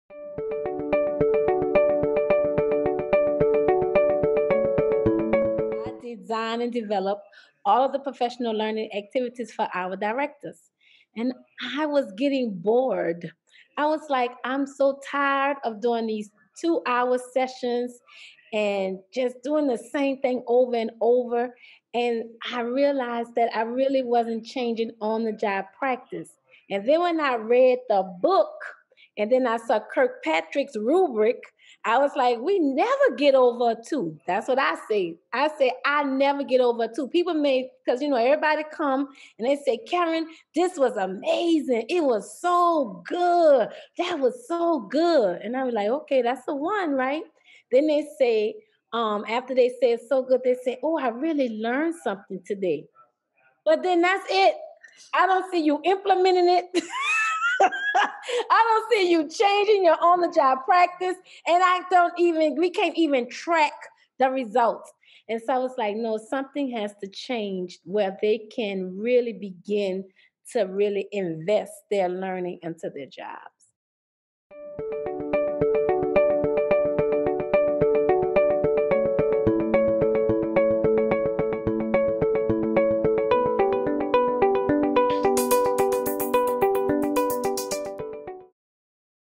What Stands Out About the Learning Cluster Design Model?
Karen Bryan-Chambers, Senior Director of the Leadership Academy at New Leaders, shares why the Learning Cluster Design Model stood out to her.